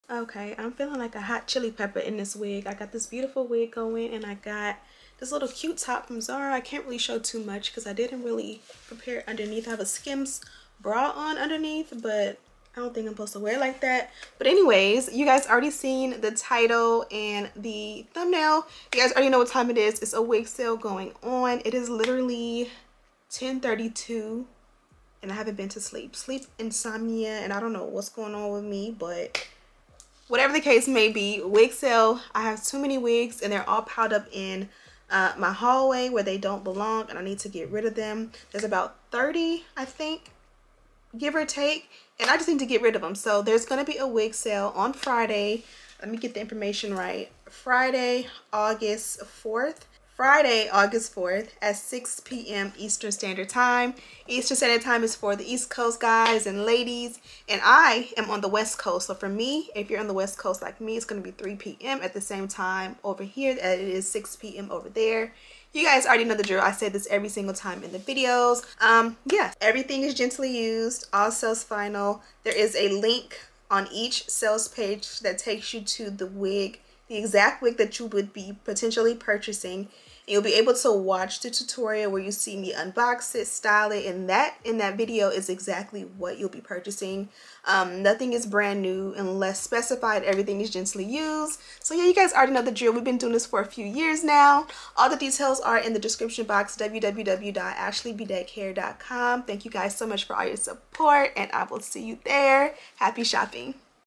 WIG SALE TODAY AUGUST 4, 2023 6pm (EST) | 30 GENTLY USED WIGS FOR SALE!
All Items Are Gently USED!

TODAY! AUGUST 4, 2023 @ 6:00 pm Eastern Time
3:00 pm West Coast Time
4:00 pm Mountain Time
5:00 pm Central Time
Please! Please! Please! Google What Time Zone You Live In!

No Password Needed... Website will open for everyone at date and time of launch!

  **U.S. Only**

-SMOOCHES 💜💜💜